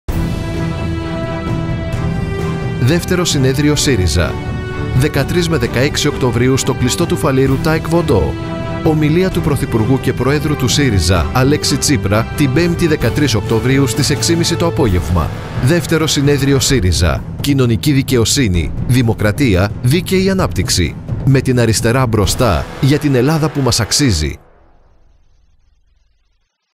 2o Συνέδριο ΣΥΡΙΖΑ: Με την Αριστερά Μπροστά για την Ελλάδα που μας αξίζει
2o Συνέδριο ΣΥΡΙΖΑ
13 με 16 Οκτωβρίου
στο Κλειστό Φαλήρου

Ομιλία του πρωθυπουργού και προέδρου του ΣΥΡΙΖΑ Αλέξη Τσίπρα την Πέμπτη 13 Οκτωβρίου στις 18:30

2ο Συνέδριο ΣΥΡΙΖΑ
Κοινωνική Δικαιοσύνη – Δημοκρατία – Δίκαιη Ανάπτυξη

Με την Αριστερά Μπροστά
Για την Ελλάδα που μας αξίζει